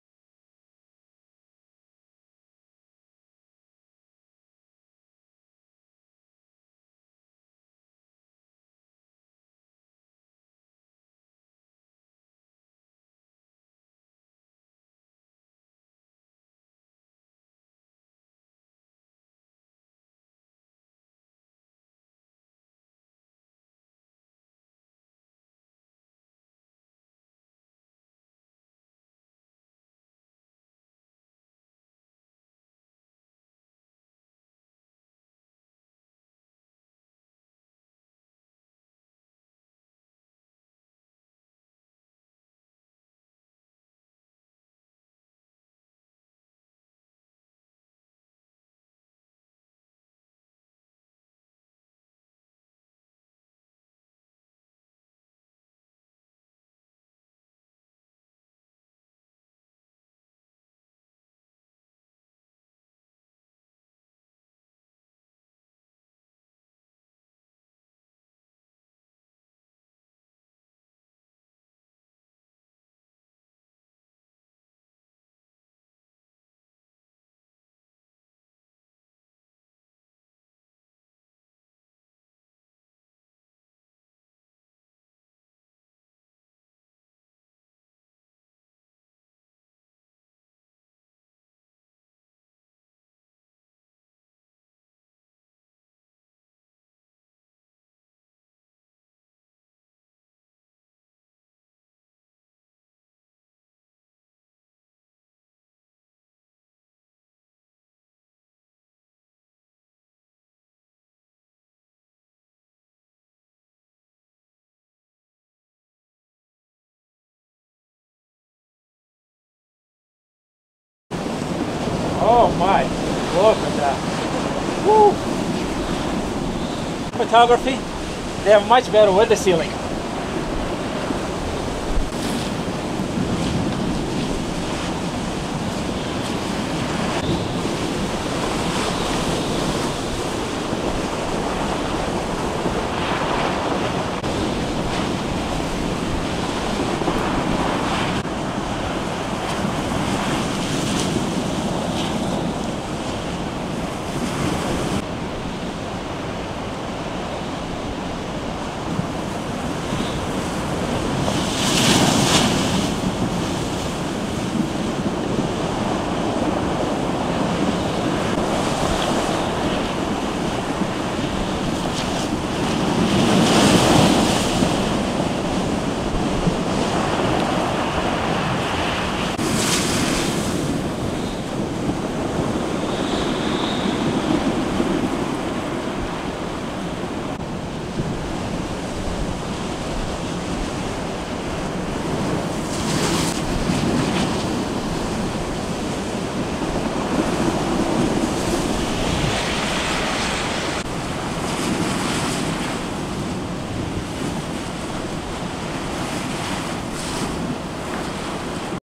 10-27-2021 Scituate, Ma drone video of massive waves slamming seawall
Drone shots of big waves in Scituate, Ma  duing the high tide this afternoon during the nor'easter.